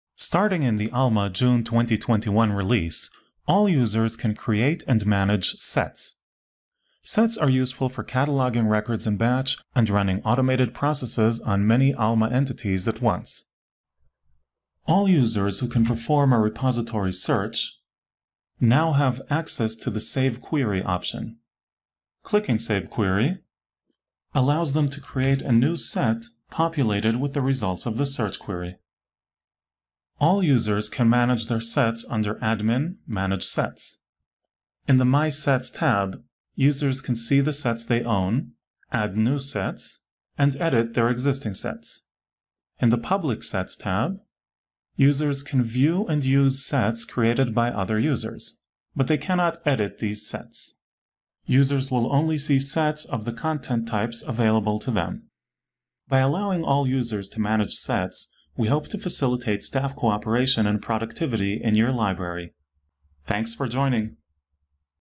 Alma June 2021 Release: All Users Allowed to Manage Their Own Sets
Starting in the Alma June 2021 release, all users can create and manage Sets. Sets are useful for cataloging records in batch and running automated processes on many Alma entities at once.